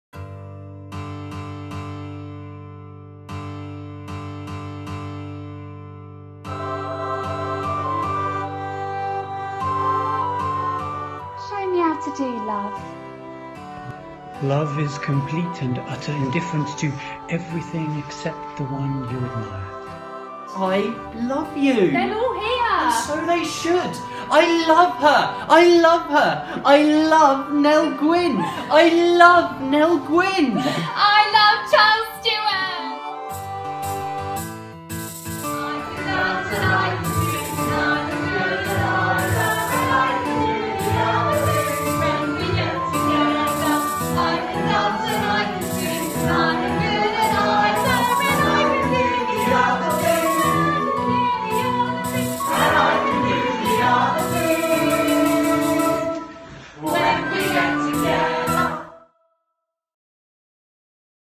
Nell Gwynn Trailer 3
Final trailer for our March 2017 production, Nell Gwynn. Keep eyes peeled for information on more sell out productions such as this one.